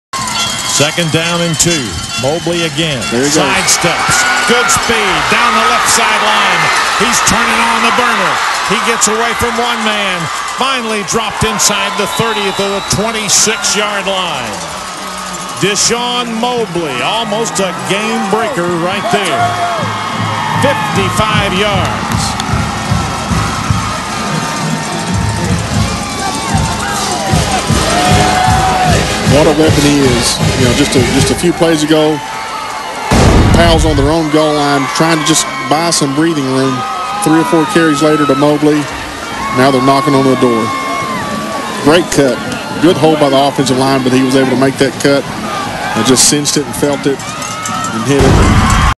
Dy'Shawn Mobley 54 Yard Rush
In the 2011 BlueCross Bowl Div. I 5A, Powell's Dy'Shawn Mobley rushes for 54 yards.